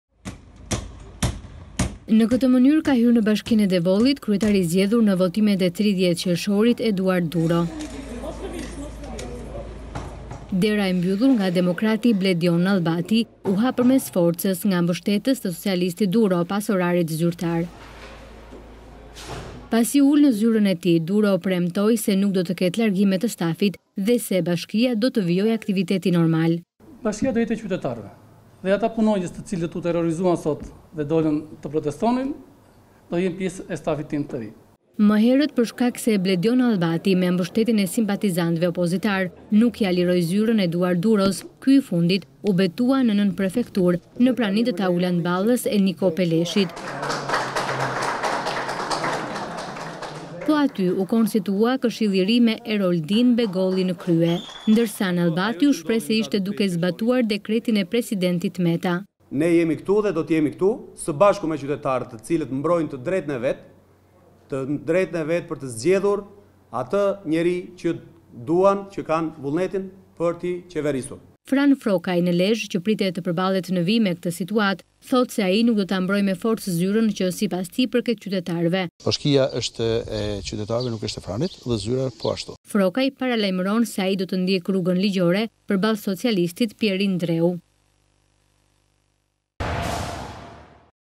RTV Ora - Devolli me dy “kryetarë”, Duro Bashkia do të jetë e qytetarëve
Në këtë mënyrë  ka hyrë në votimet e 30 qershorit Eduartd Duro. Dera e mbyllur nga demokrati Bledion Nallbati u hap përmes forcës nga mbështetës të socialistit Duro pas orarit zyrtar.

Pasi u ul në zyrën e tij Duro premtoi se nuk do të ketë largime të stafit dhe se bashkia do të vijojë aktivitetin normal.

Duro: Bashkia do të jetë e qytetarëve dhe ata punonjës të cilët u terrorizuan sot dhe dolën të protestonin do jenë pjesë e stafit tim të ri.

Më herët për shkak se Bledjon Nallbati me mbështetjen e simpatizantëve opozitarë nuk ia liroi zyrën Eduard Duros, ky i fundit u betua në nënprefekturë në prani të Taulant Ballës dhe Niko Peleshit.

Po aty u konstituua edhe Këshilli i Ri me Eroldin Begolli në krye, ndërsa Nallbati u shpreh se ishte duke zbatuar dekretin e presidentit Meta.